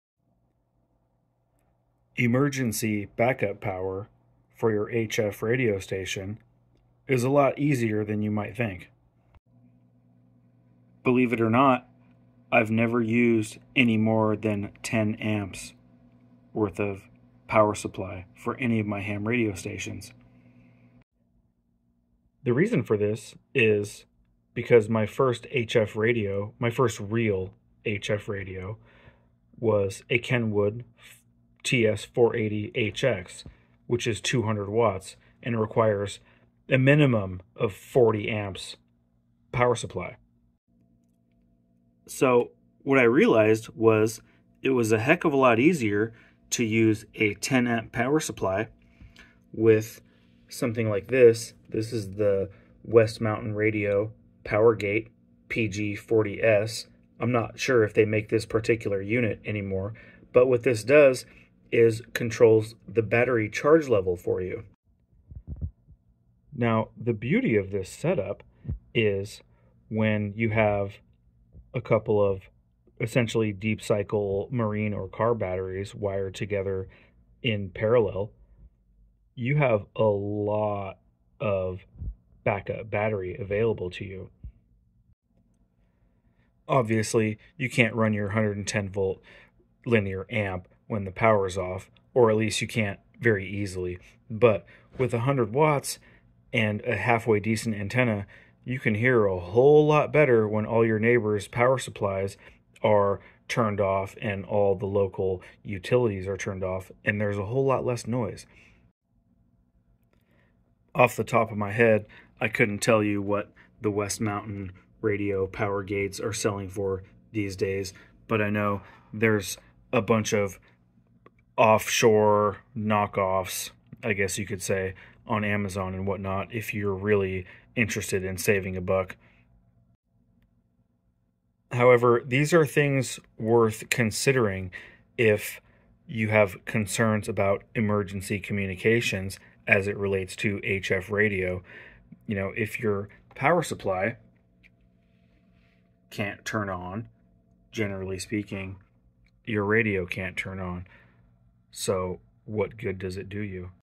GRID DOWN SHTF RADIO SHACK EZ MODE BATTERY BACKUP SYSTEM - KEEP TALKING WHEN THE LIGHTS ARE OUT
An easy method for ensuring emergency battery backup for your HF ham radio station.

With only a 10 amp power supply and one or more car batteries, you can keep your ham radio station running seamlessly in a blackout or grid-down situation. Rather than rely on a 110v power supply to keep your communications in tact, consider using a charge controller to float charge some batteries, and run your station from the batteries.

This ensures you've got backup power no matter the circumstances in just about any type of emergency situation